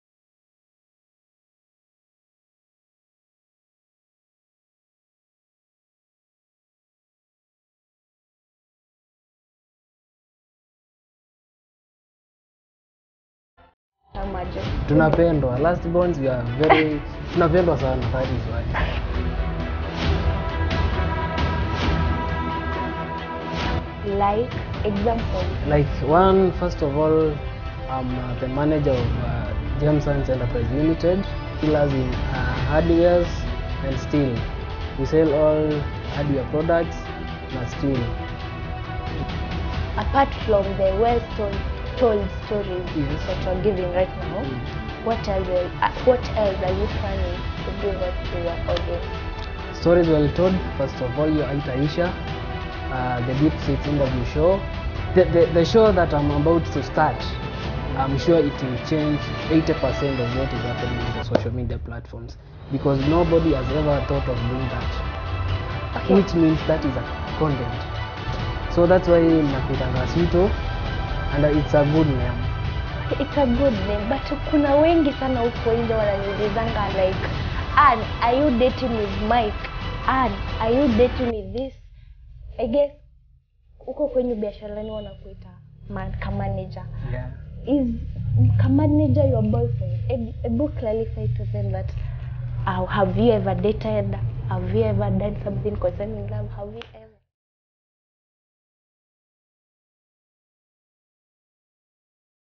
ASWITTOO ACCUSES A.BOY OF DATING MULTIPLE LADIES | I'M NOT A BOYFRIEND SNATCHER | STOP MOCKING ME😭.
Mike_AfricanBOY-kenya is a robust youtuber hosting the Africa's No.1 "Deep-states interview show" and "Stories Well Told."